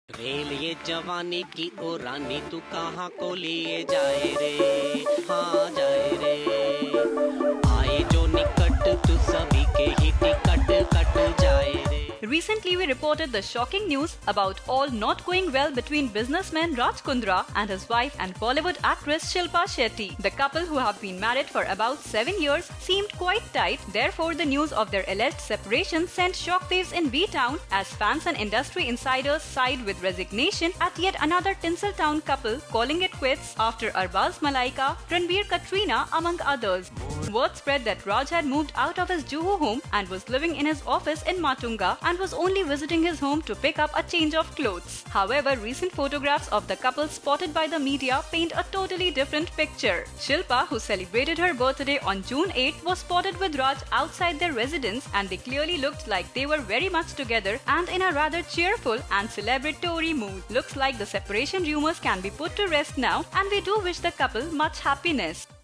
What's going on with them | Bollywood Masala | Latest Bollywood News
Yes, they are very much in love and the pictures speak volumes about that. We are talking about B Town’s one of the most adorable couple Shilpa Shetty and Raj Kundra. Well there have been rumours that both the husband and wife have parted ways and Raj is living separately in his office. But these rumours are far from the truth.